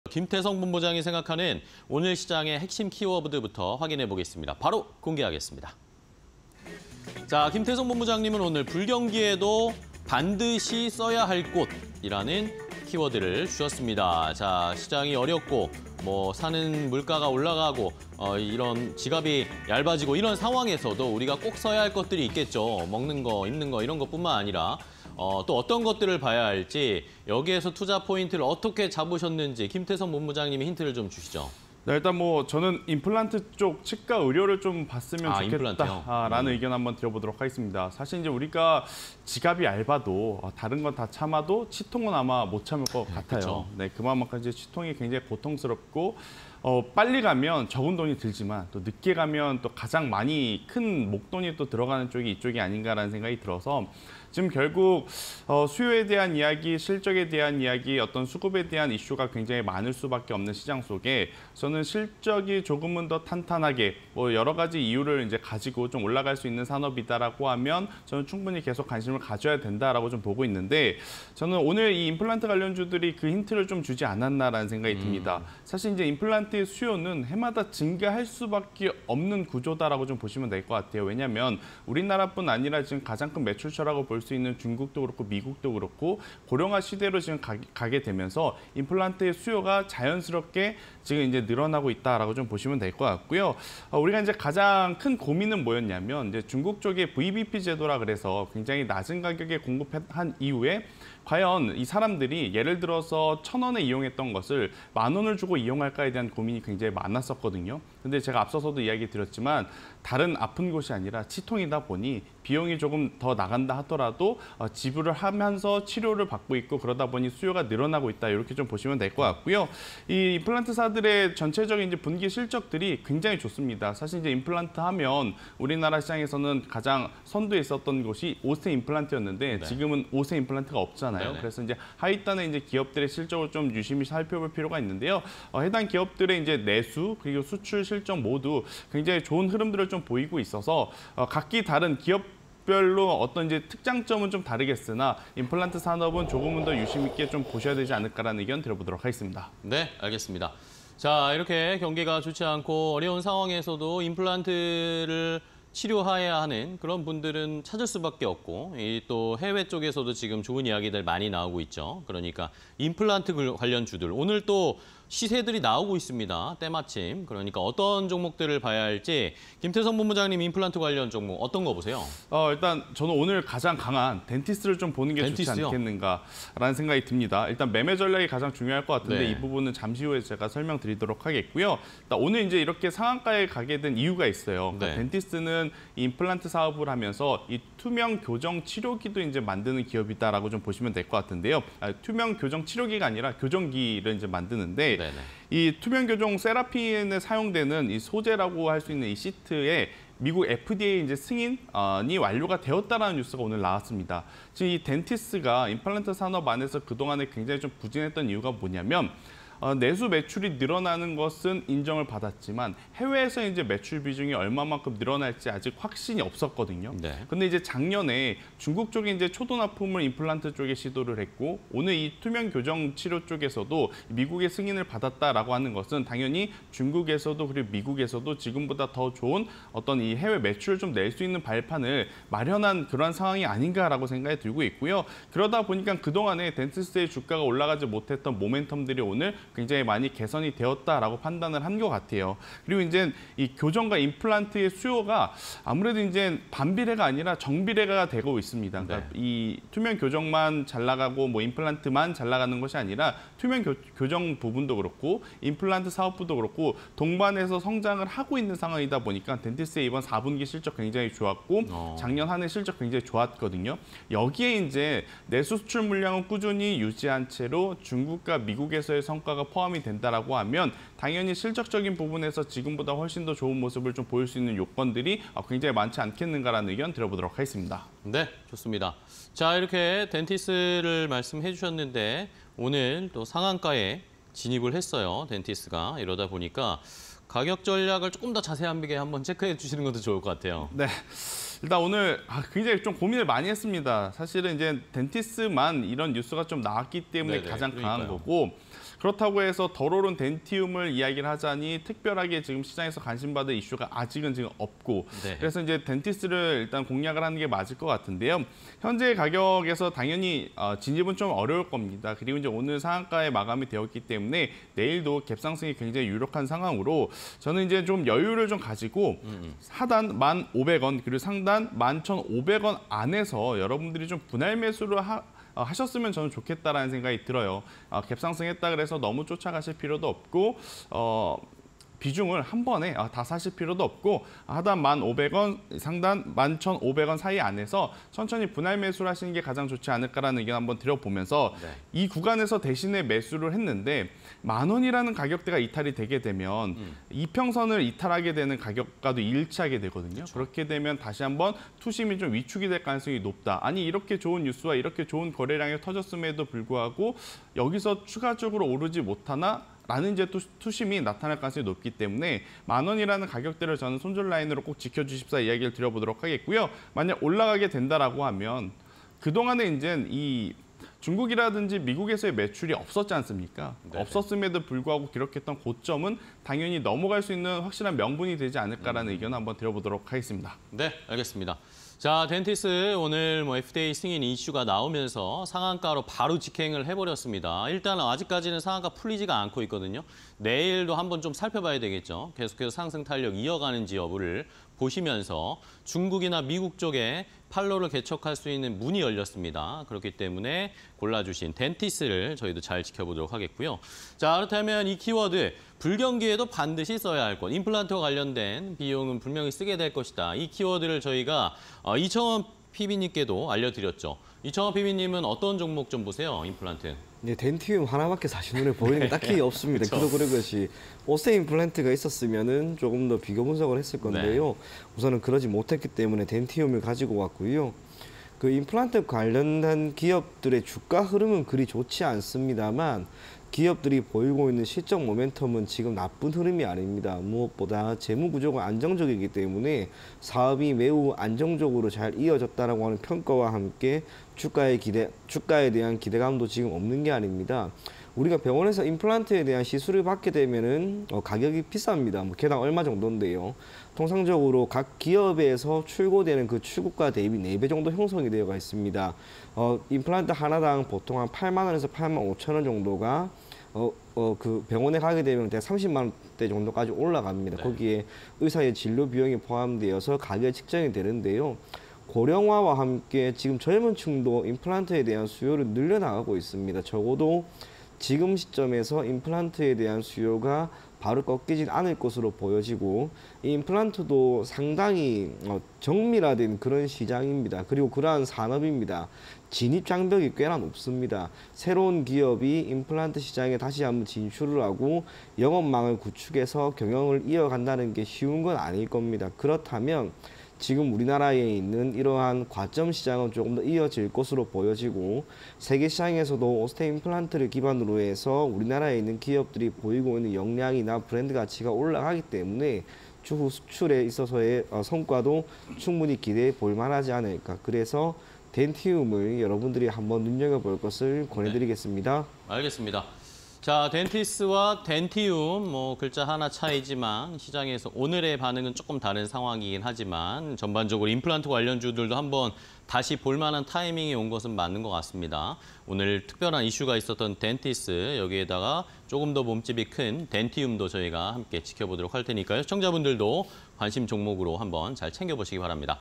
덴티스(261200), 덴티움(145720)... 임플란트_키워드 배틀 (20240228)
- 임플란트 치료 보편화... 시술 건수 급증
- 중국 현지 수요 고민 대안책 마련
- 분기 실적 호조세

덴티스(261200)
- 투명교정 시트 ‘세라핀’ 美 FDA 승인
- 해외 모멘텀 개선 전망
- 투명교정‧임플란트, 동반 성장 기대

덴티움(145720)
- 중국 진출 안정화... 매출 40% 증가
- 중국 내 임플란트 공급 가격 안정적
- 향후 실적 개선 기대